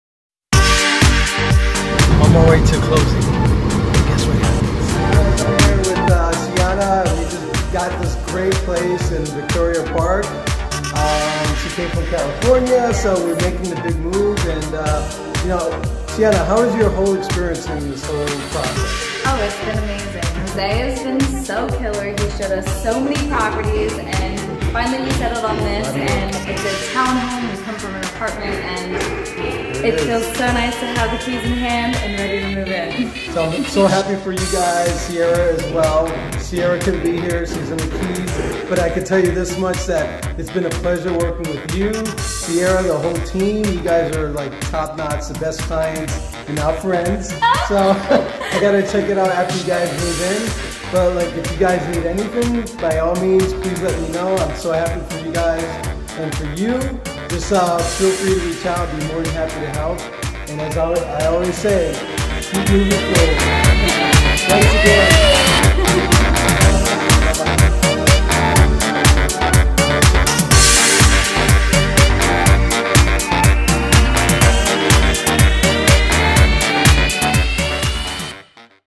Client gets the townhouse!
Clients get the townhouse!  During one of the most competitive markets in decades if not ever, this client got what they wanted all within weeks of doing showings.  So if you work hard enough in any market you can achieve success!